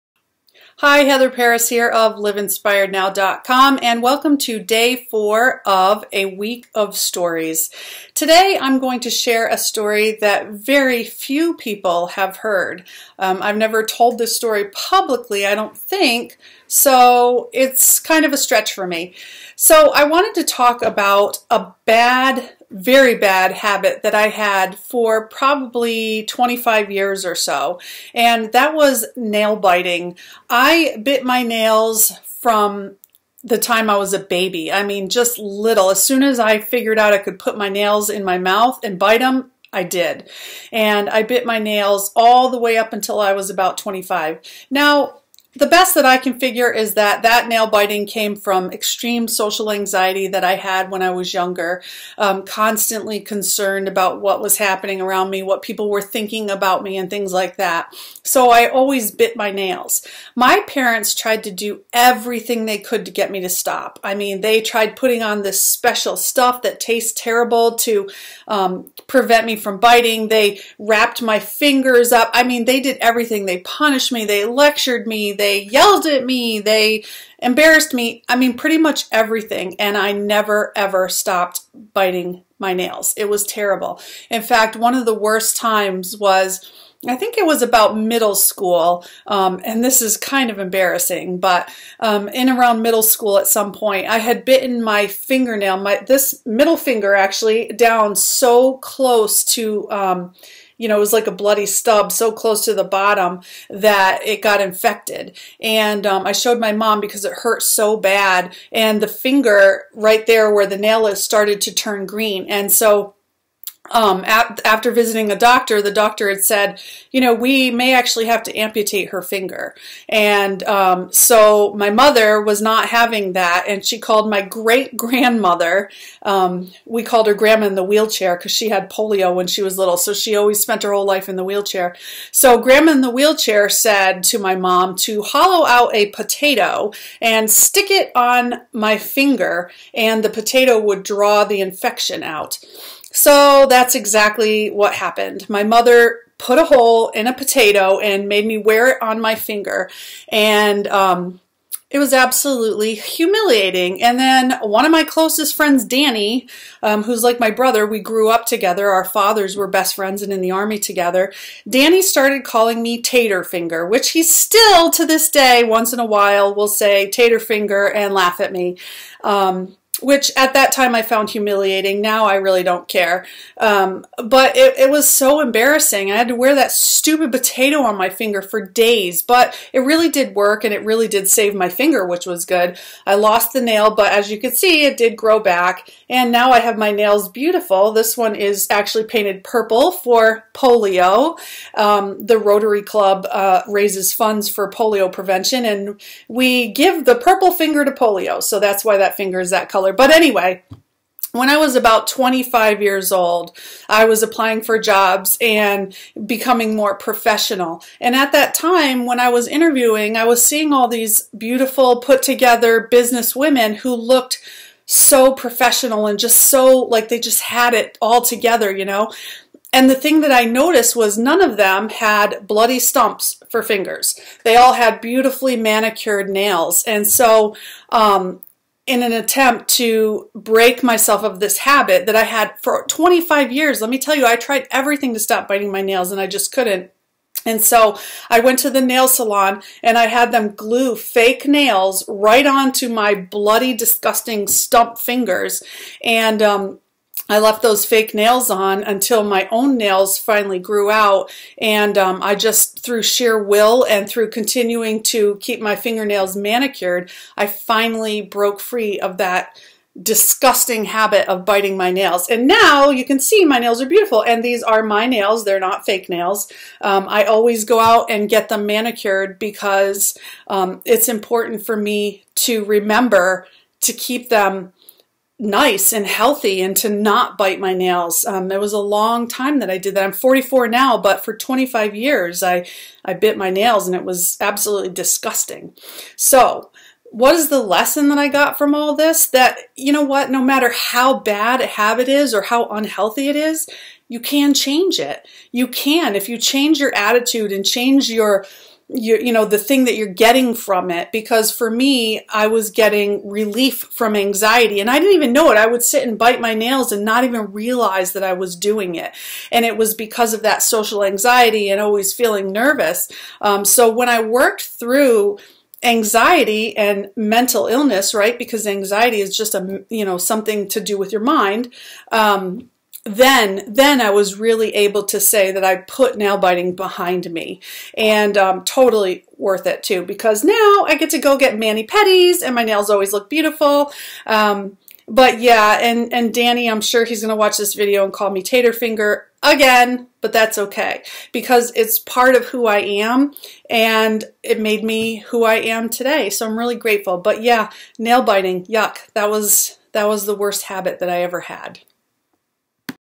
A WEEK OF STORIES Day 4: How I almost lost a finger!
Anxiety was something I secretly dealt with all of my young life. Unfortunately, I had a pretty disgusting way to cope with it.